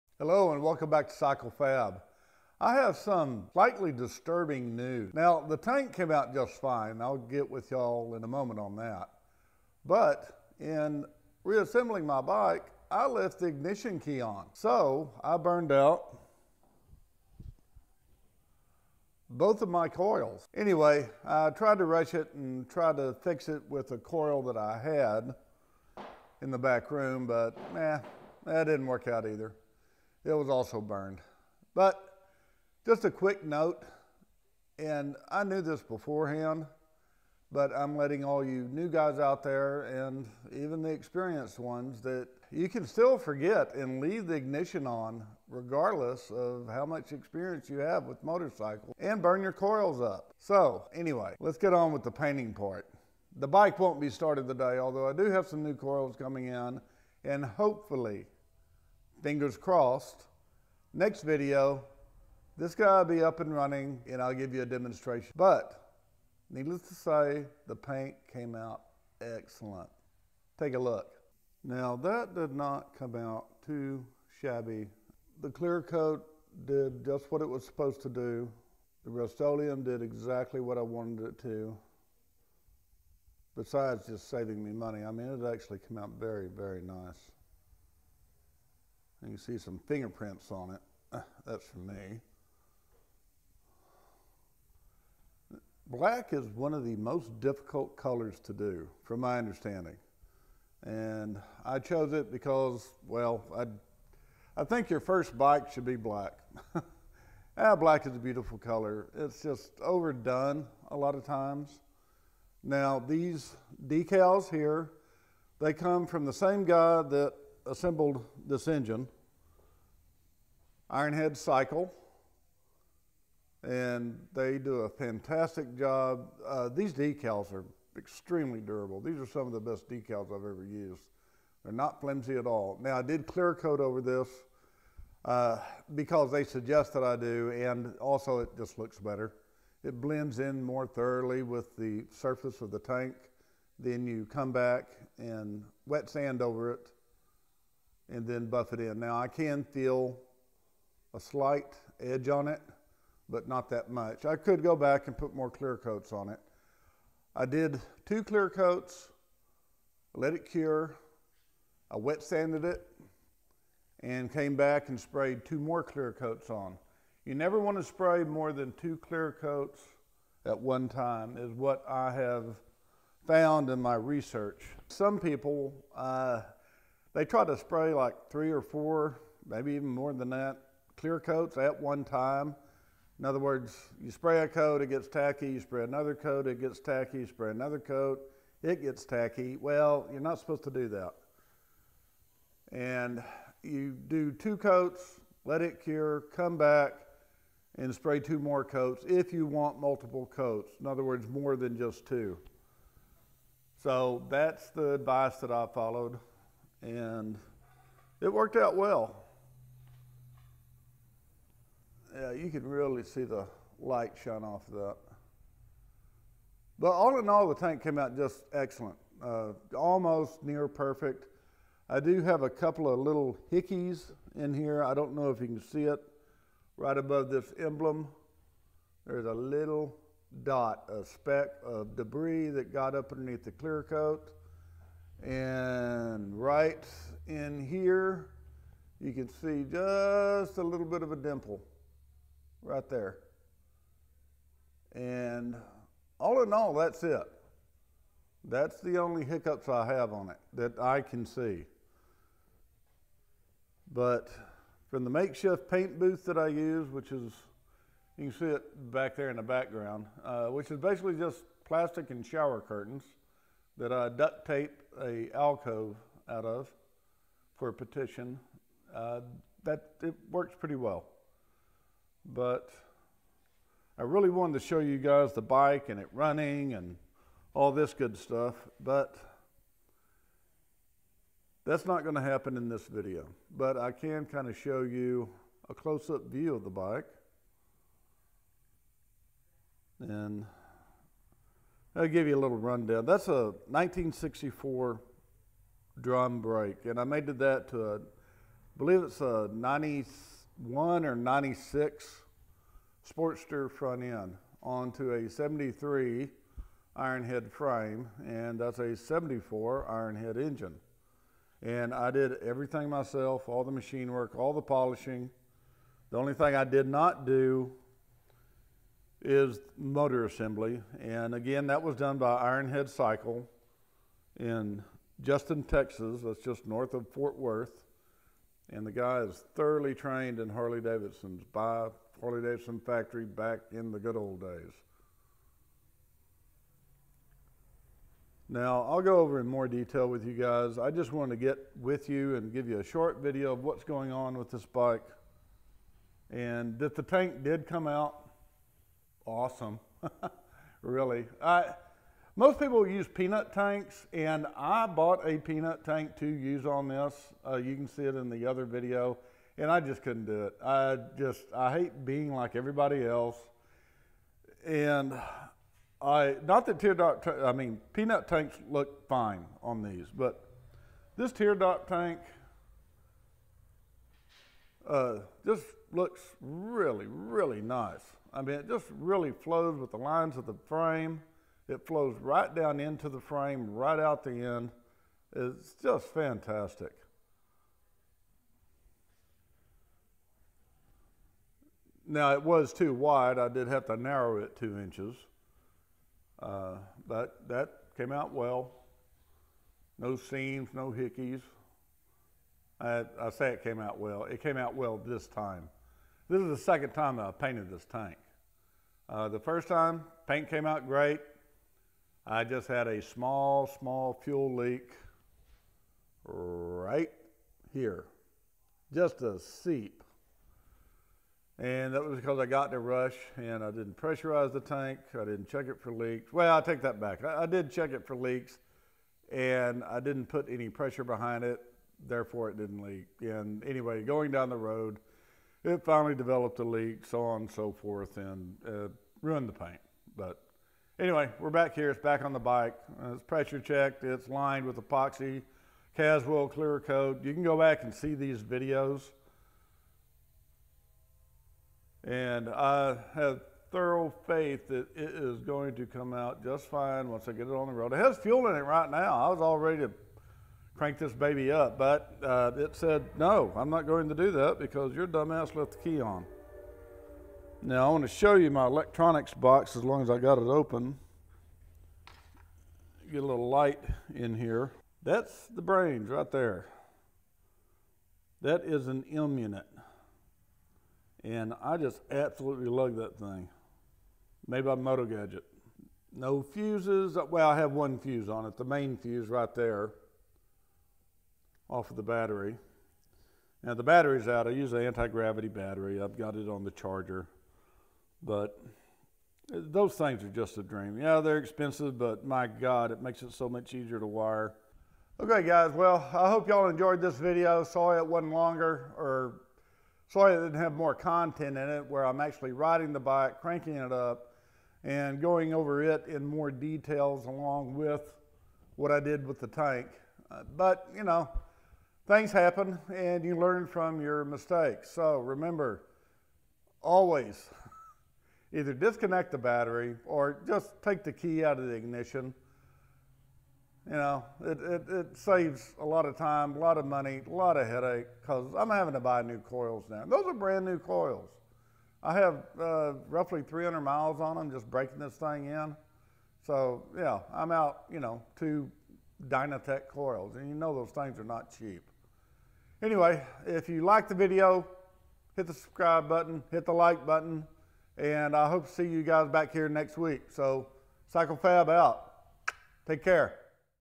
PAINTING A MOTORCYCLE WITH RUSTOLEUM (#2)
PAINTING A MOTORCYCLE WITH RUSTOLEUM (#2) can save you lots of money and still come out looking as good as professional paint.